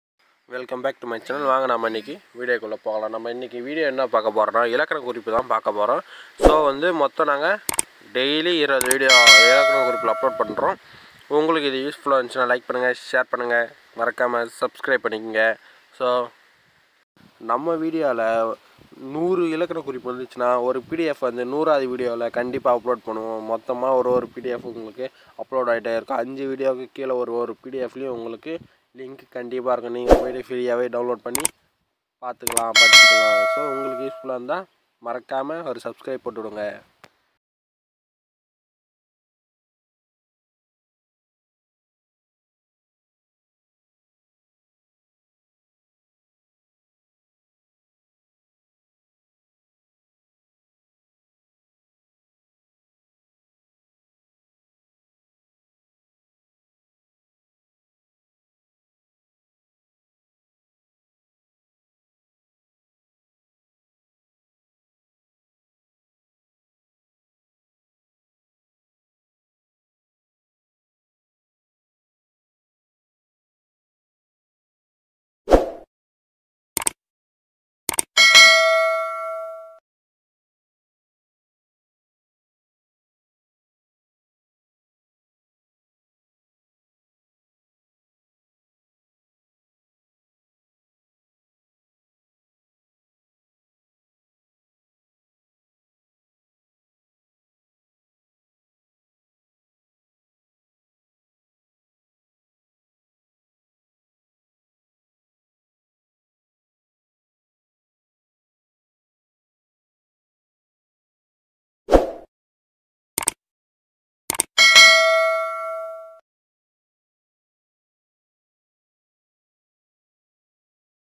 தமிழ் இலக்கணம் tnpsc day 3
Tnpsc Group 4 and 2

தமிழ் இலக்கணம்
Day 3

PDF DOWNLOAD 👇 LINK IN
100 இலக்கணம்
Day 5 video in download 👇 link தரப்படும்

💥தமிழ் இலக்கணம் day 1

💥 தமிழ் இலக்கணம் day 2